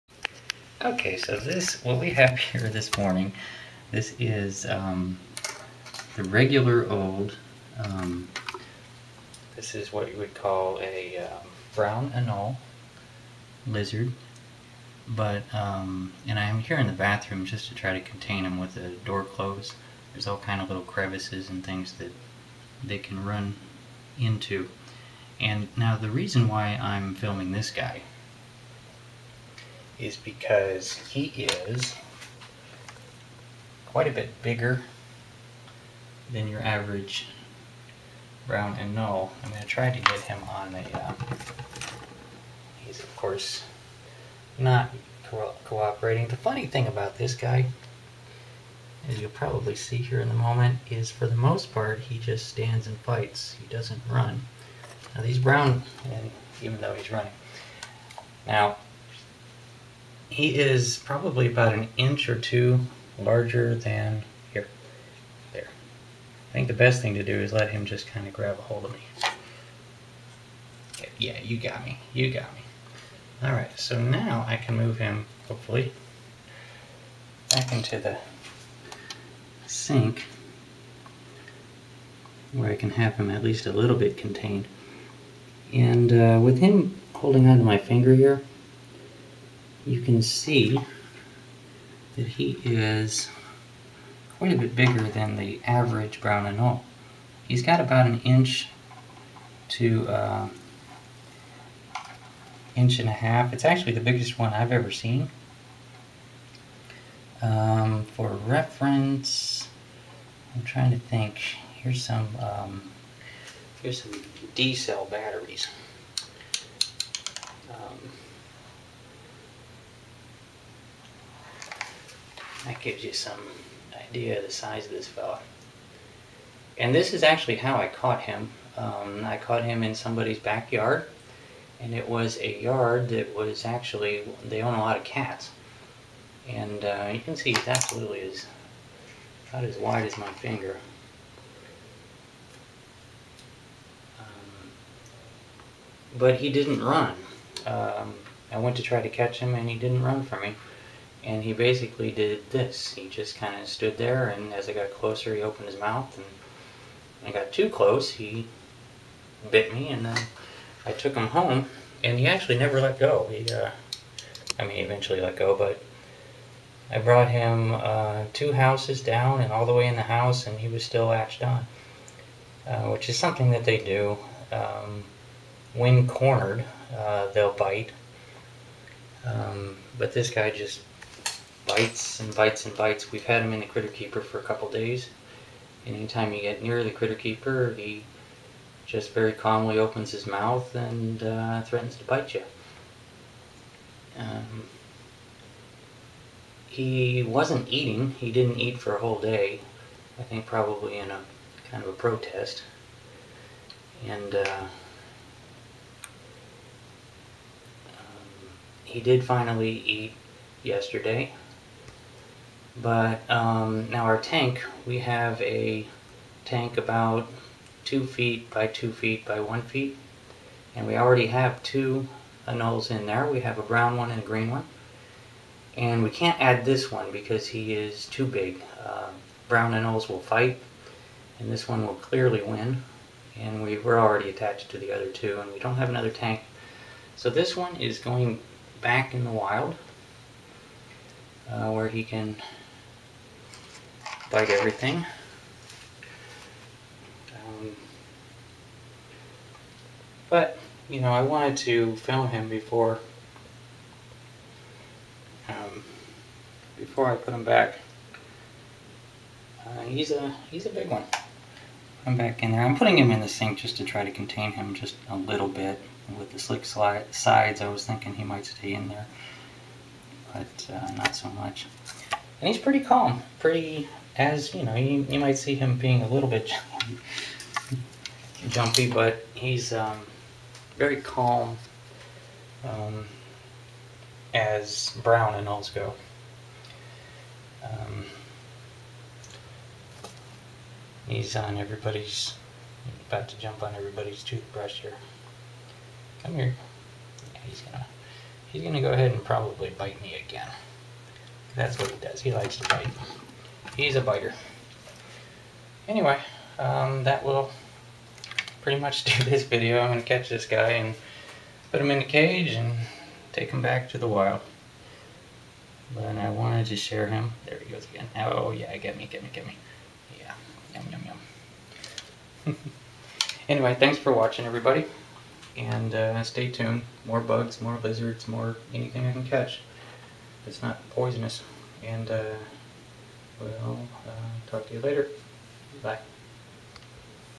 Big Brown Anole Lizard
This is a very big brown Anole lizard found in Orlando, FL.